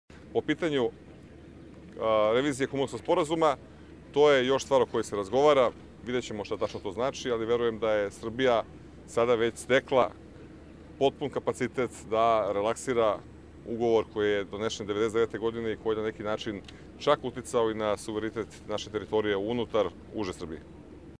Otvaranje mosta u selu Mazarać - Novinarsko pitanje - O Kumanovskom sporazumu
Ministar odbrane Dragan Šutanovac i načelnik Generalštaba Vojske Srbije general Miloje Miletić posetili su danas opštinu Vladičin Han i tom prilikom otvorili most na Južnoj Moravi u selu Mazarać. Ministarstvo odbrane i Vojska Srbije